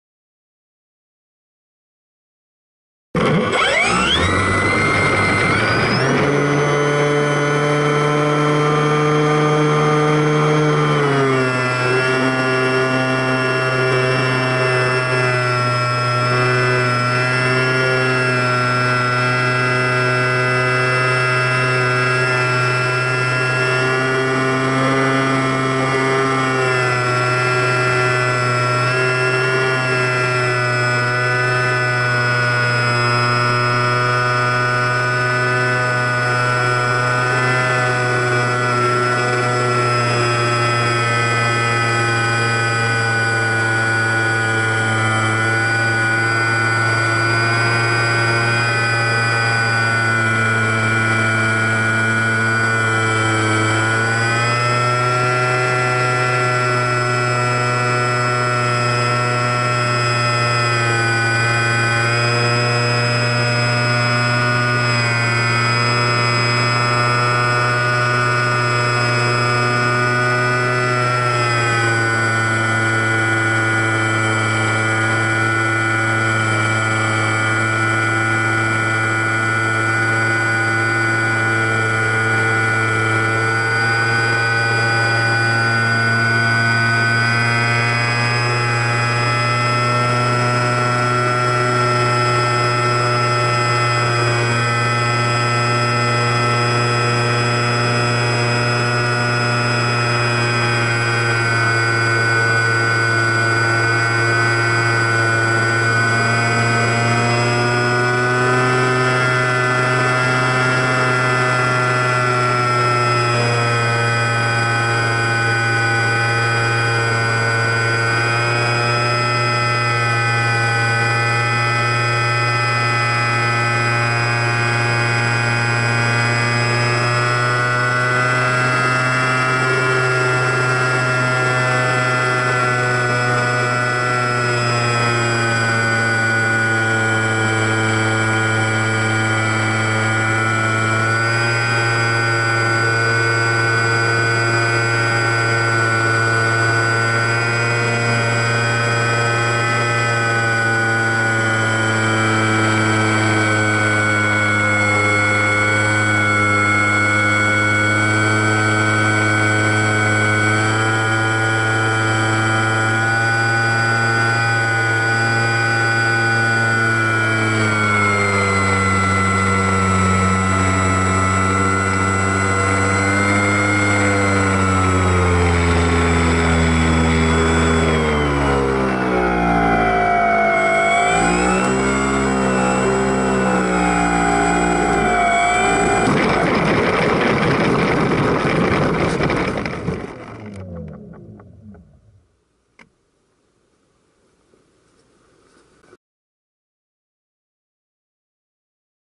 ラジコン飛行機 Cockpit view.
ラジコン飛行機コックピットビュー　Sbach342にアクションカムHDR-AS100Vを載せてみました。
キャノピー越しだと画像がボケる。
手振れ補正ON　1080p60
翌日、フライト練習でストールターン後ににアンプが燃えて、まっすぐ地面に墜落・・・
この動画が最後のフライト映像となってしまいました。